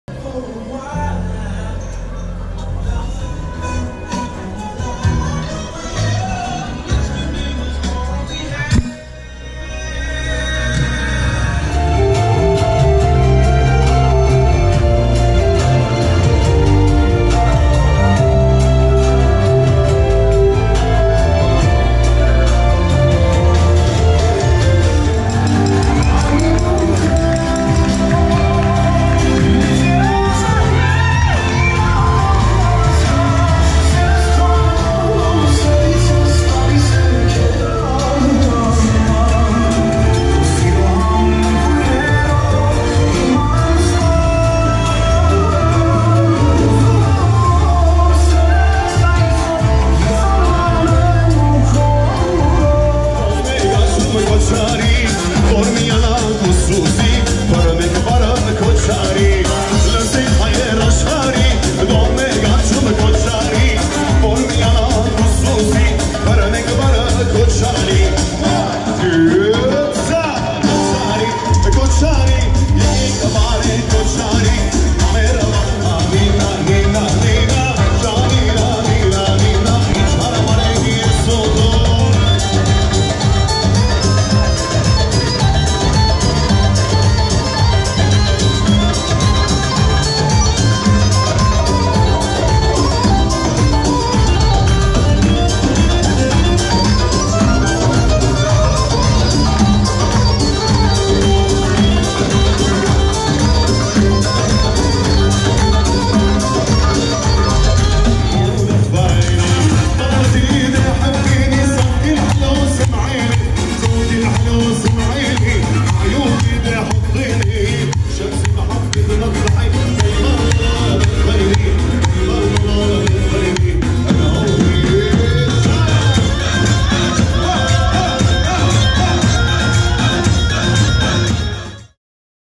Embassy Plaza / Sako Tashjian /Dj Youssef / Hovo / Armenian Wedding / Montreal
singer : Sako Tashijian
Armenian Keyboard : Hovo
Dj Youssef
Lighting And Sound : Youssef bardakji